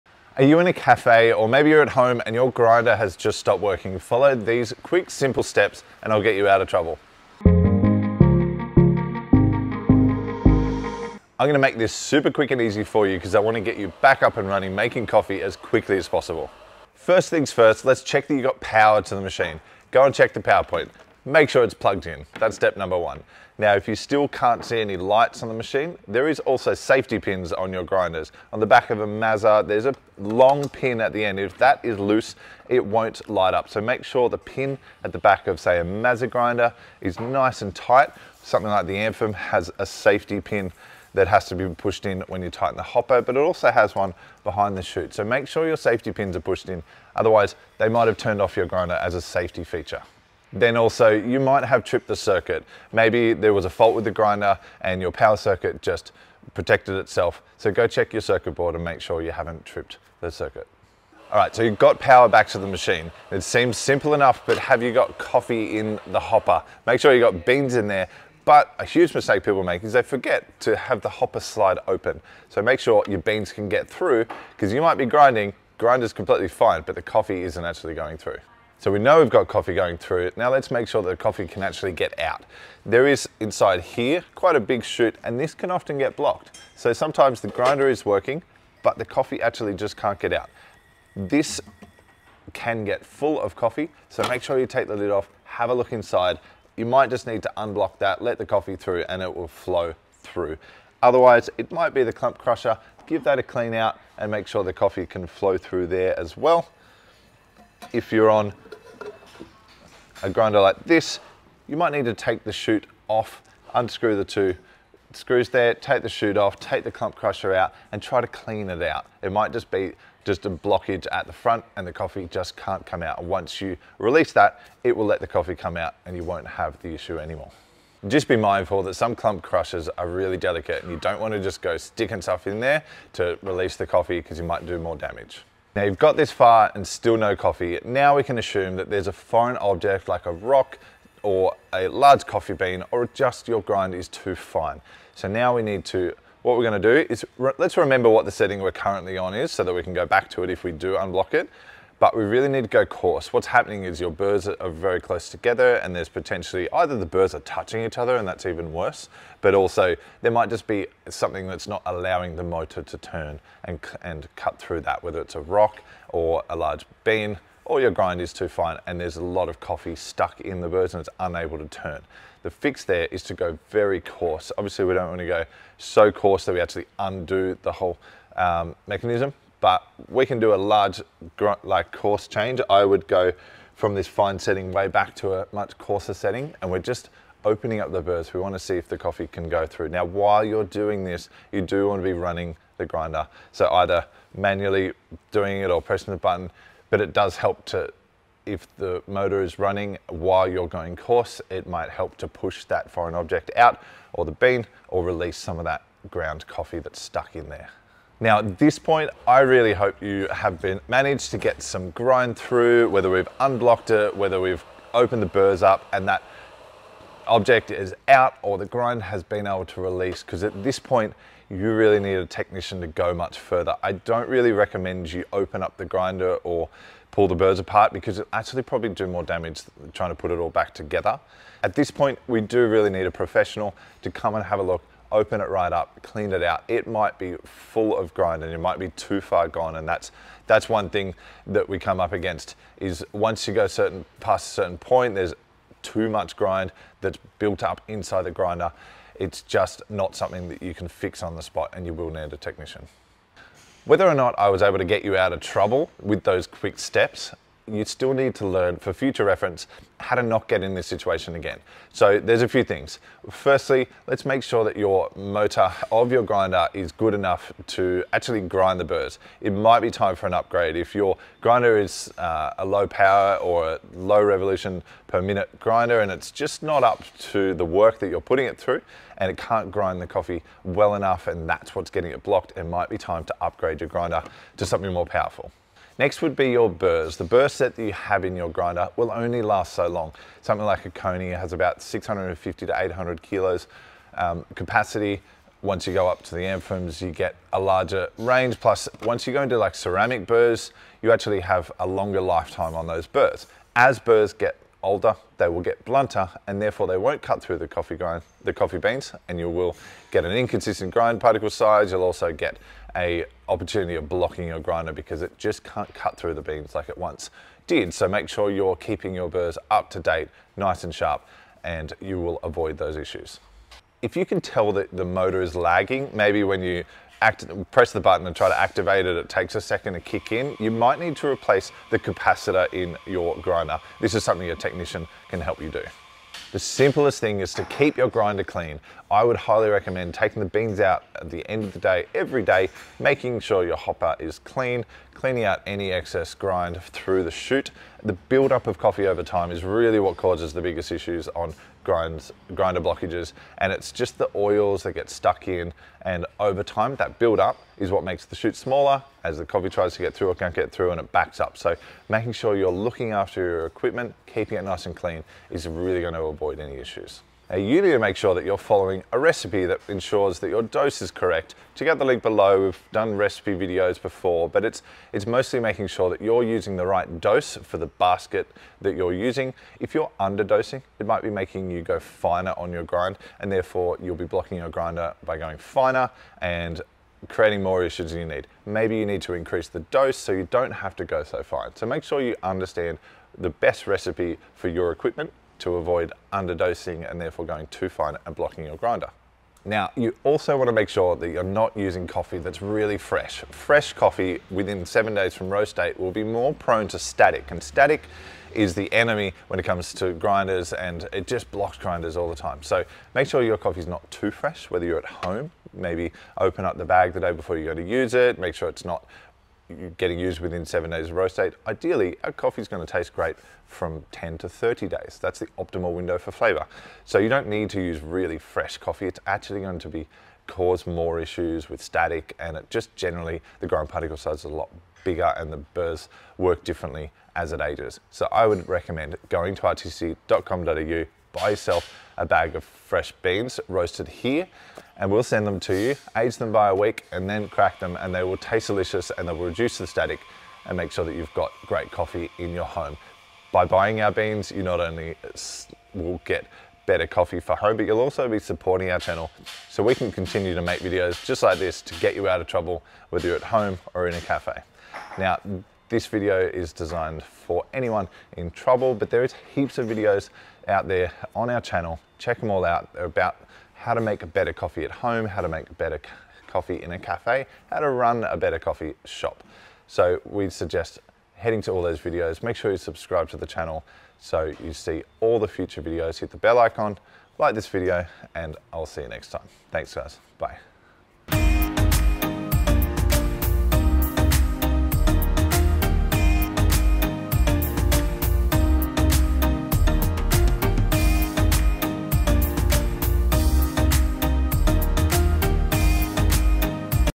How to Fix Coffee Grinder Problems (Fastest Solutions)
Are you facing a coffee emergency? Is your grinder refusing to cooperate, leaving you without your daily caffeine fix? Don't worry; we've got your back! In this super quick and easy video, Jimmy will guide you through simple troubleshooting steps to get your coffee grinder up and running again, whether you're at home or in a cafe.

We hope these troubleshooting tips save the day and get you back to enjoying the perfect cup of coffee in no time.

Time Stamps

0:00 Intro
0:03 Your grinder just stopped working, and we've got the solution!
0:14  Check Power
0:28  Check safety pins
0:48  Check your circuit board.
1:02  Beans in the hopper, and the slide is open.
1:26 - Check and clean the chute.
1:47 - Clean the clump rusher.
2:05 - Go coarser to open up the burrs.
3:57 - Foreign object or a worn-out grinder part?
4:23 - Check the grinder's motor, burrs, and capacitor.
6:04 - Use the right dose.
6:38 - Use coffee that's not too fresh.